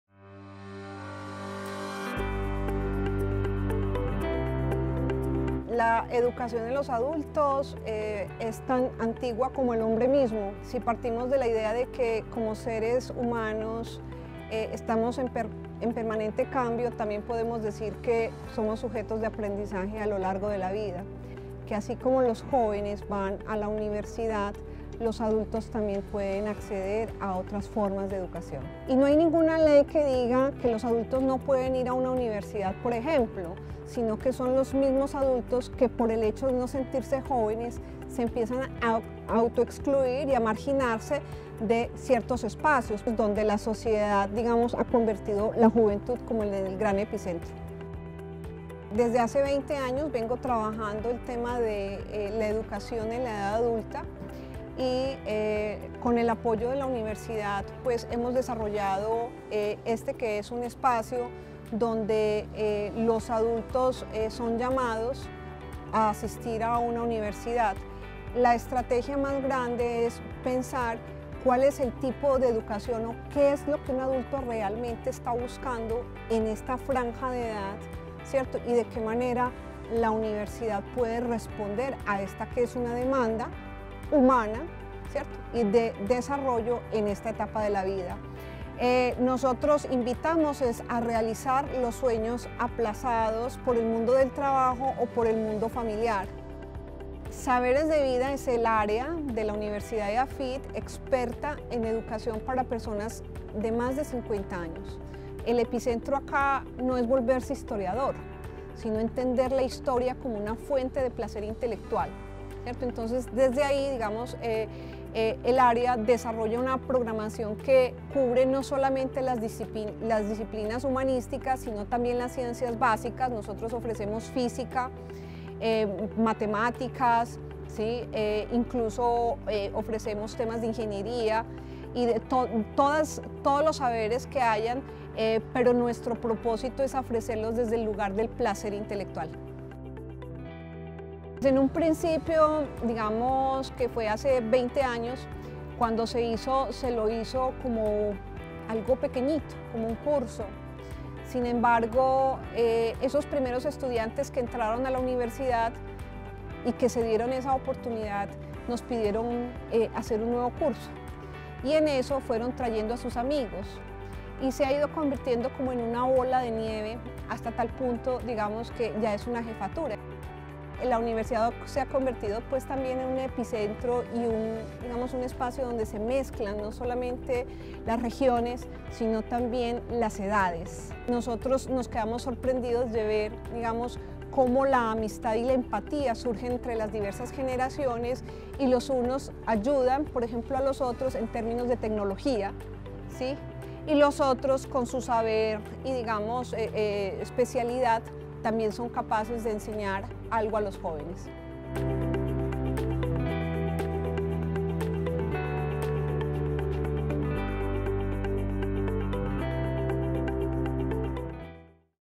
Educación sin edad: programa Saberes de vida. Revista Comfenalco Antioquia
El aprendizaje y la educación en la adultez.